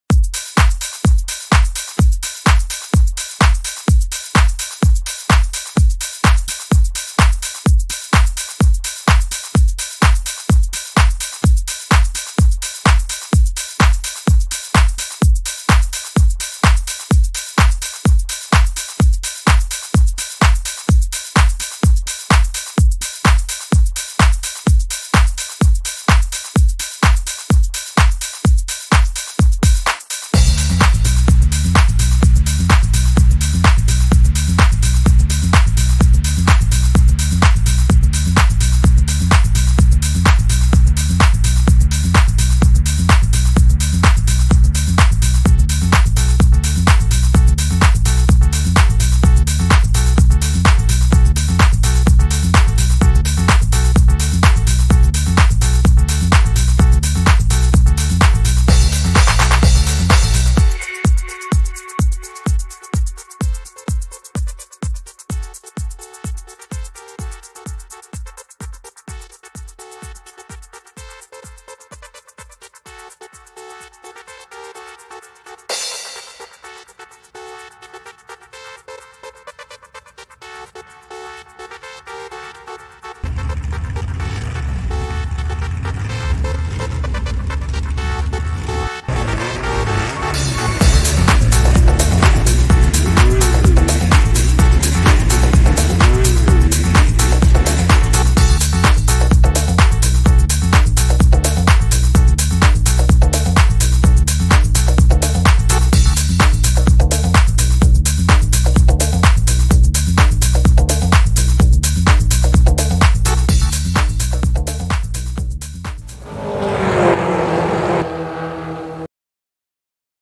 Silverstone Festival 2023
A video showcasing the Classic Racing machines of days gone by...

Silverstone Festival has become a family event full of entertainment alongside Motorsport machines, drivers and teams from back when Adam was a driver 😉😉😉💪🏼💪🏼💪🏼🏁🏁🏁🏁